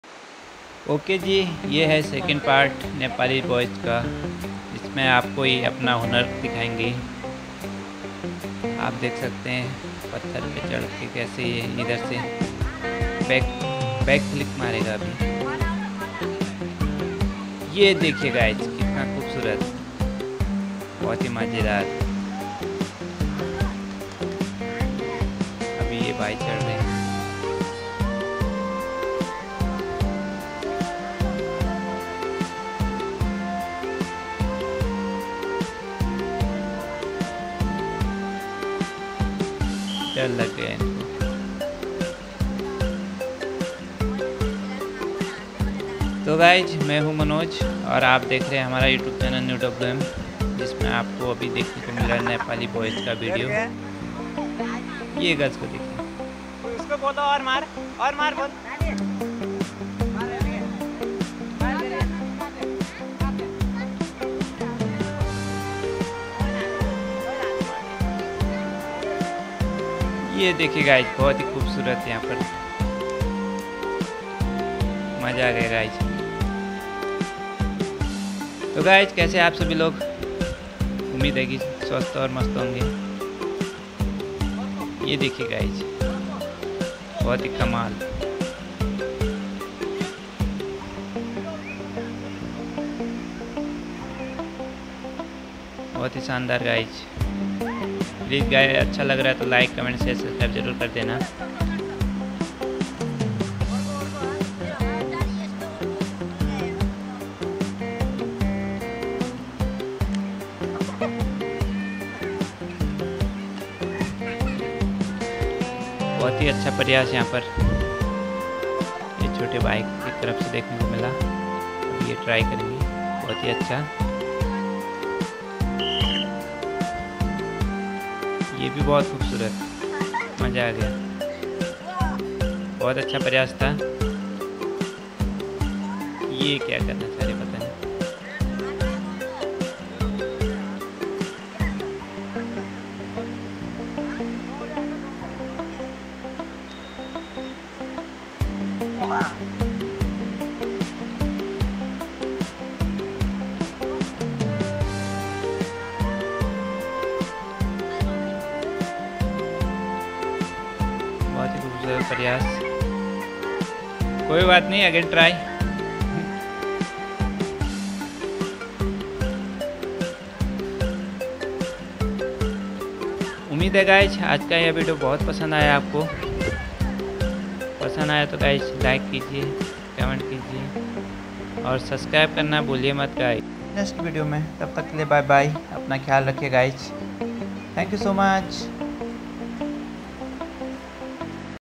Nepali Boys Trying To Backflip In The Goa Beach | New WM
hy guys I'm manoj now here new summer backflip video so guys please watch fully video
thanks